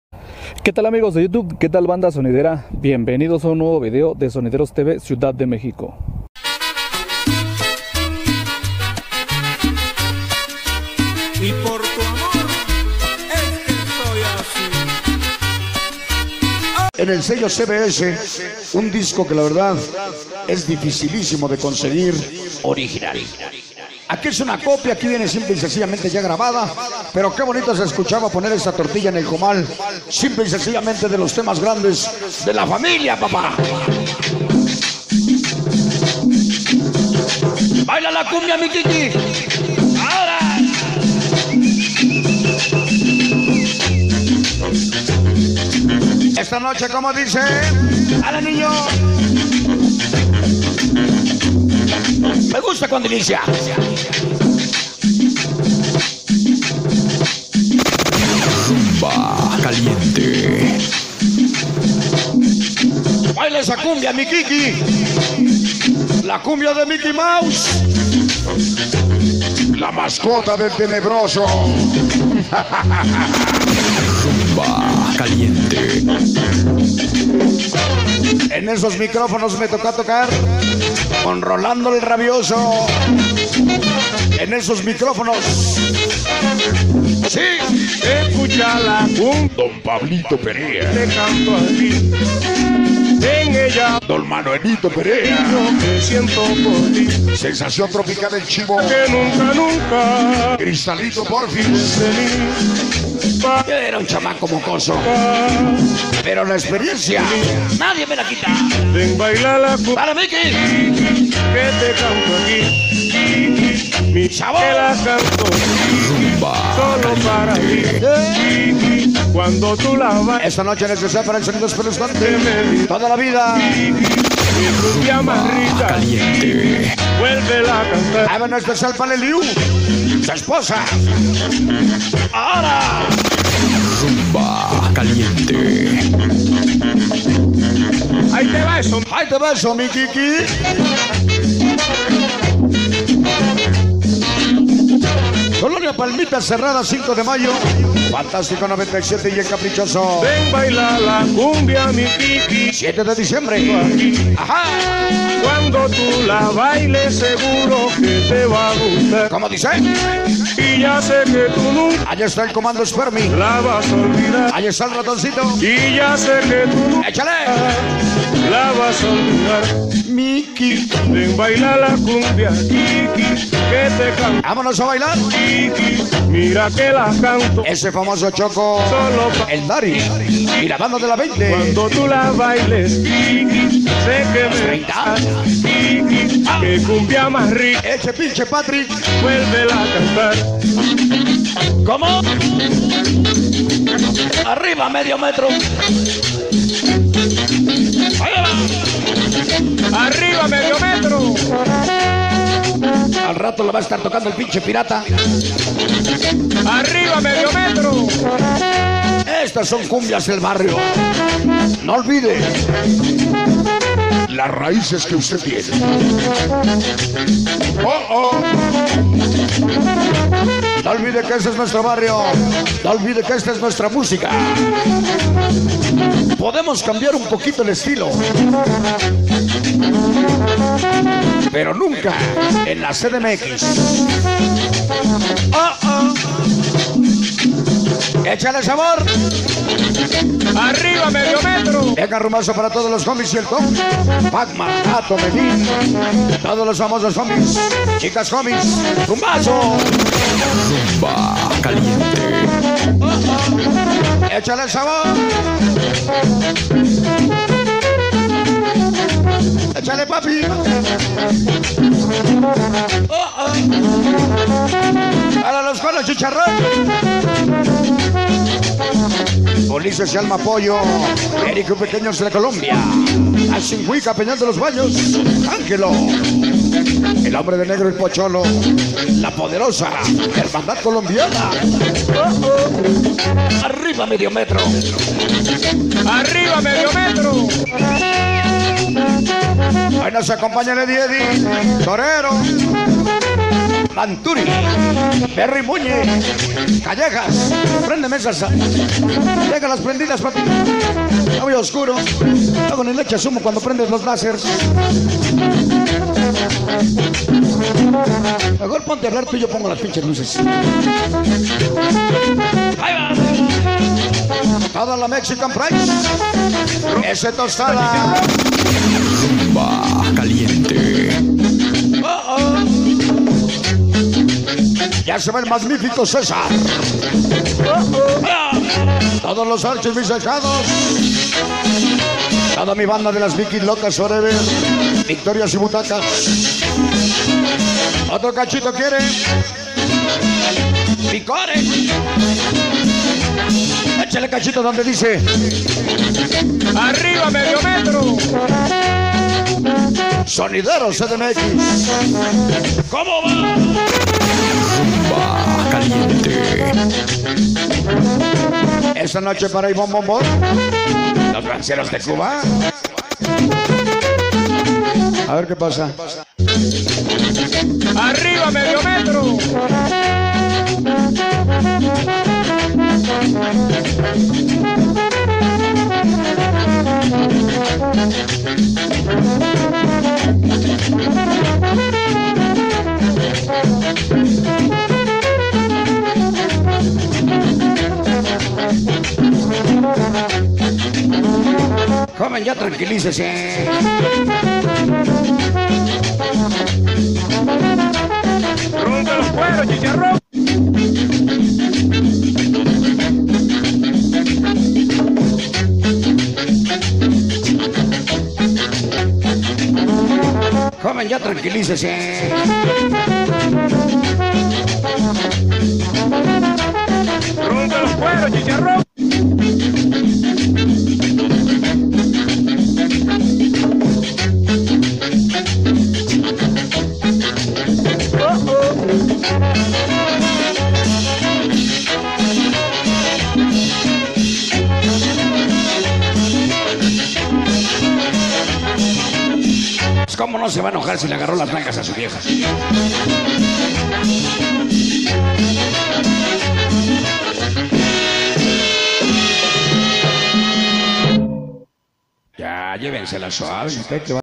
bailes sonideros FECHAS SONIDERAS DEL 18 19 20 DE ENERO DEL 2024 AGENDA SONIDERA
primer video de fechas sonideras de esta semana.

bailes sonideros de la Ciudad de México y Estados circunvecinos.